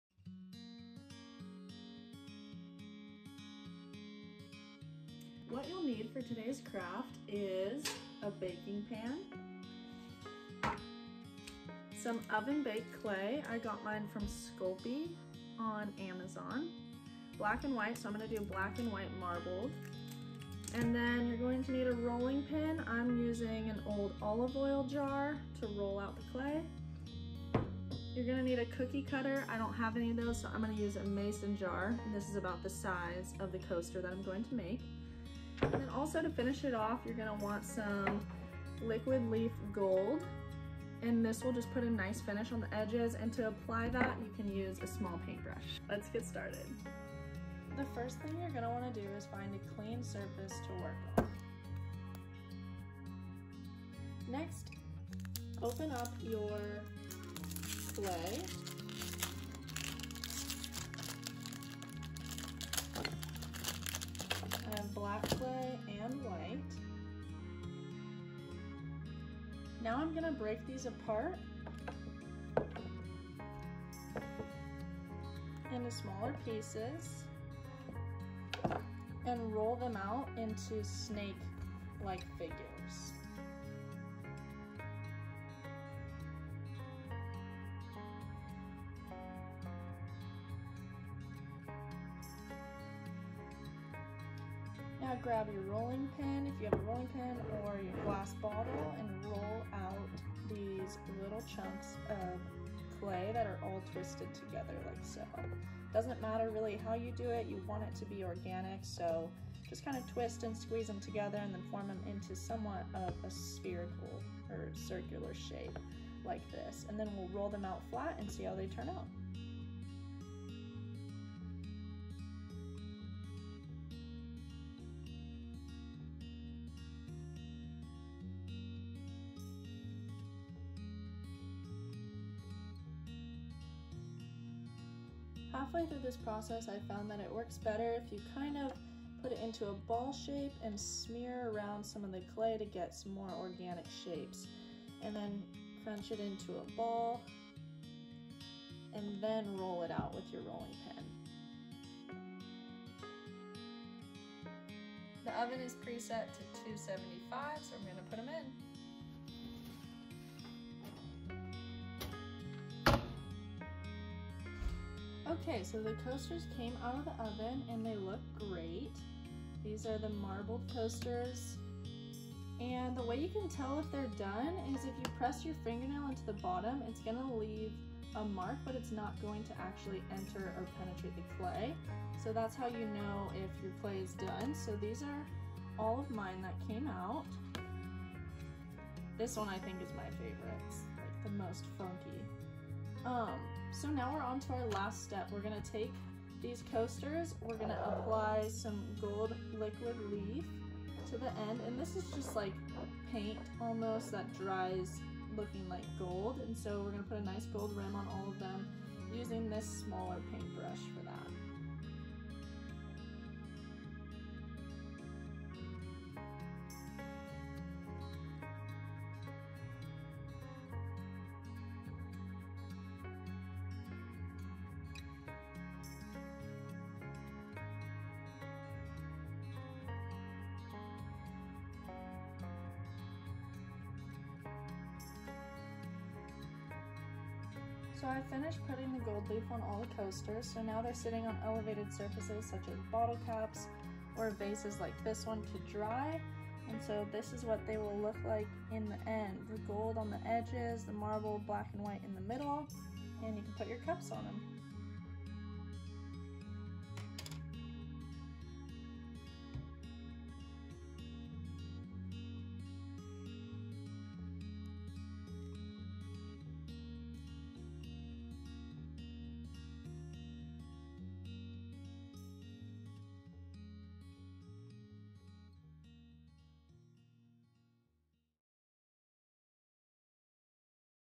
Ceramic Coasters
Make your own coasters out of clay and gold leaf to add some personality to your space!